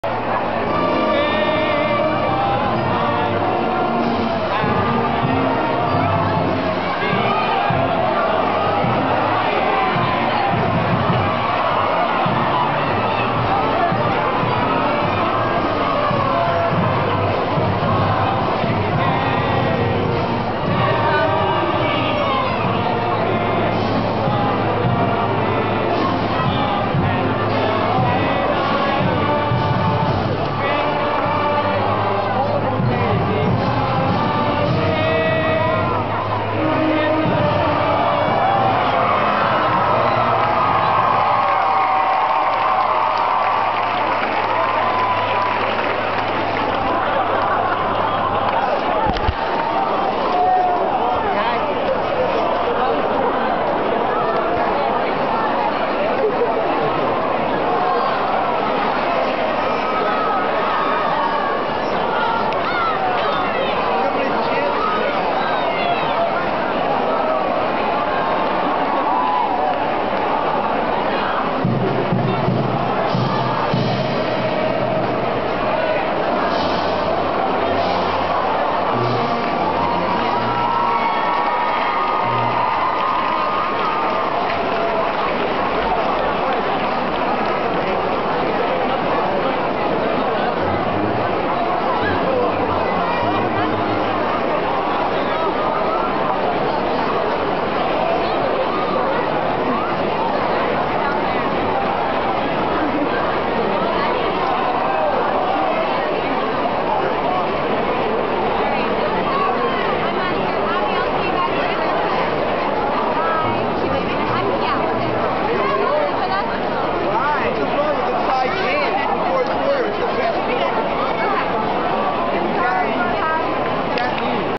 Penn Football Toasting
tradition after the 3rd quarter of play